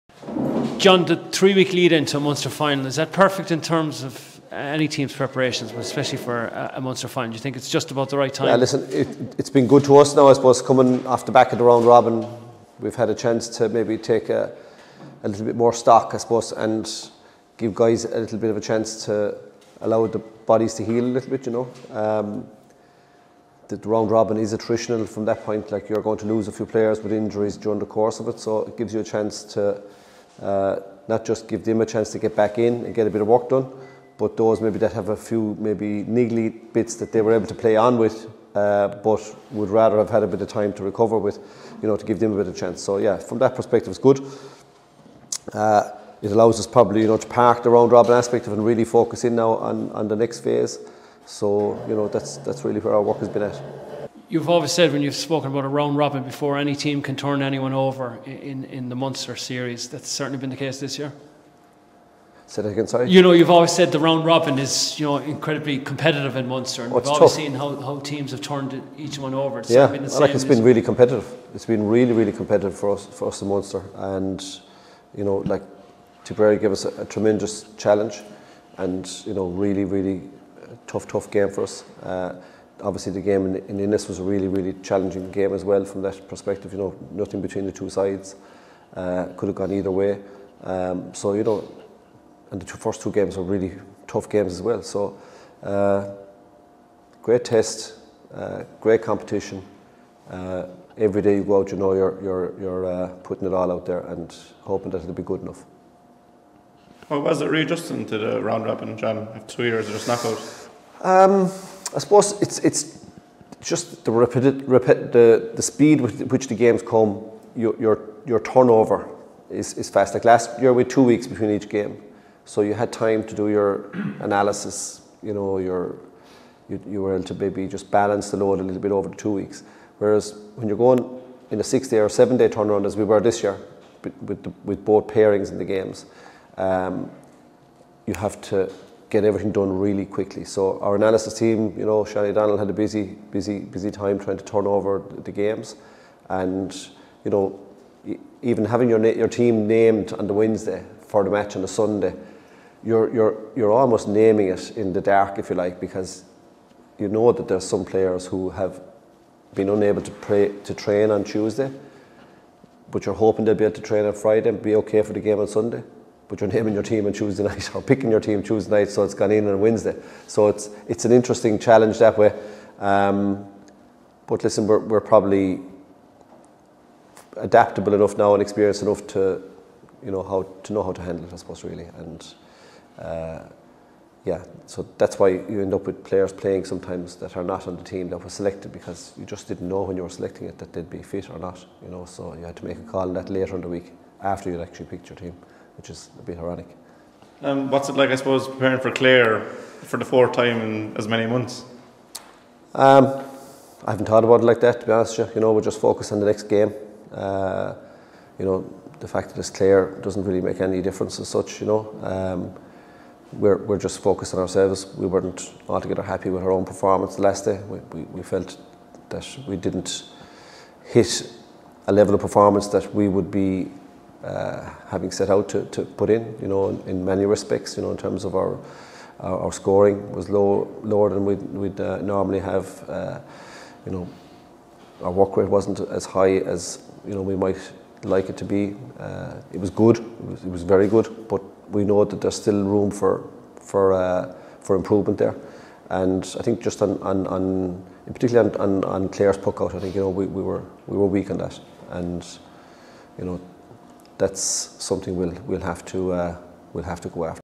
John Kiely expects a closely fought battle with Clare
John Kiely expects a closely fought battle with Clare in Sunday's Munster Final showdown and singles out areas for improvement.

"The game in Ennis was a really challenging with nothing between the two sides... we know there is still room for improvement"